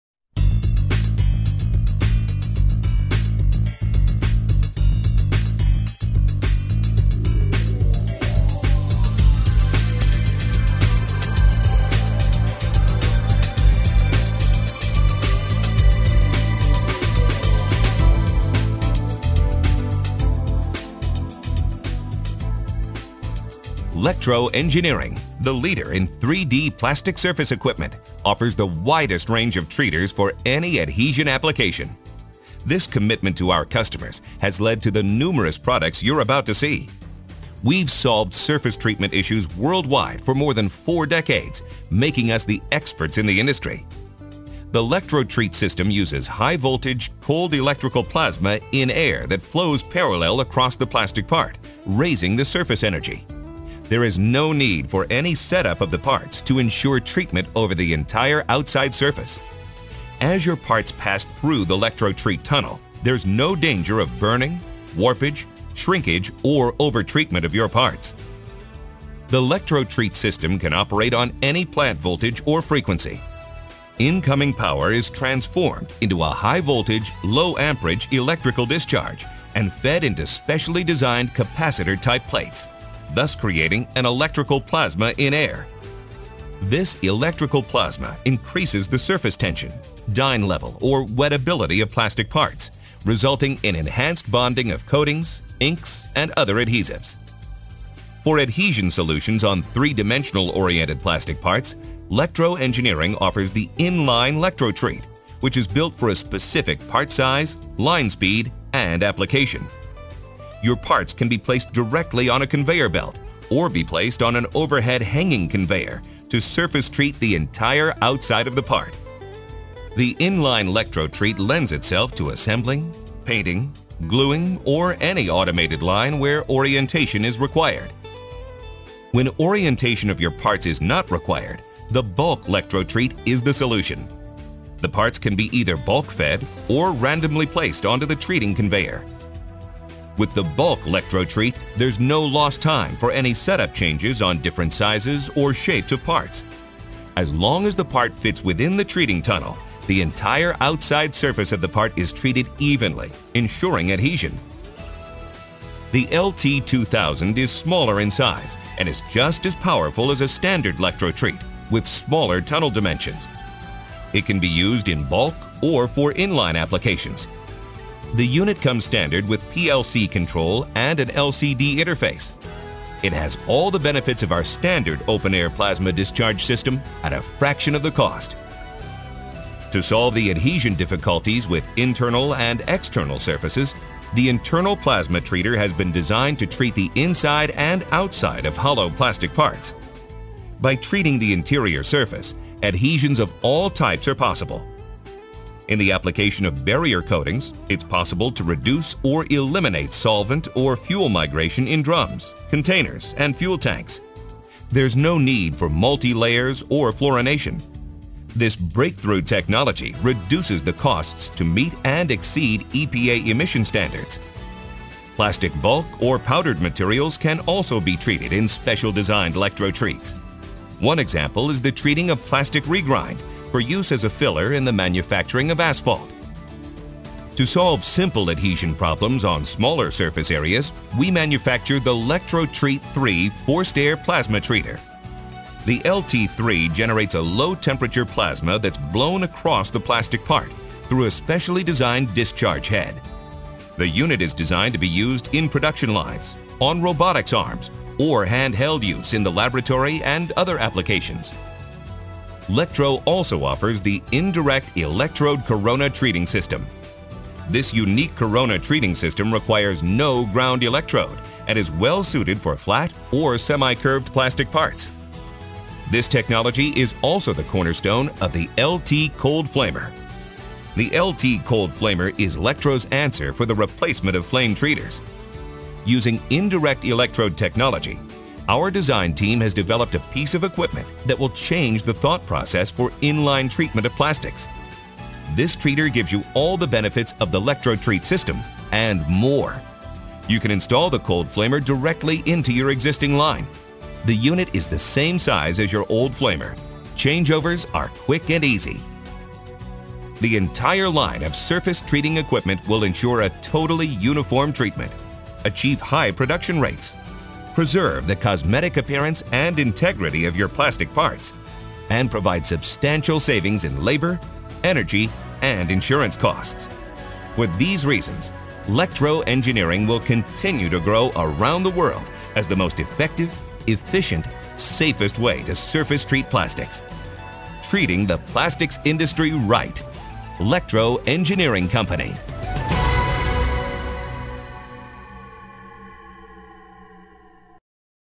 Lectro Engineering Co. Lectro-Treat Product Line
Lectro Engineering can surface treat almost any type of plastics to promote adhesion.  Lectro has a wide variety of surface treating equipment.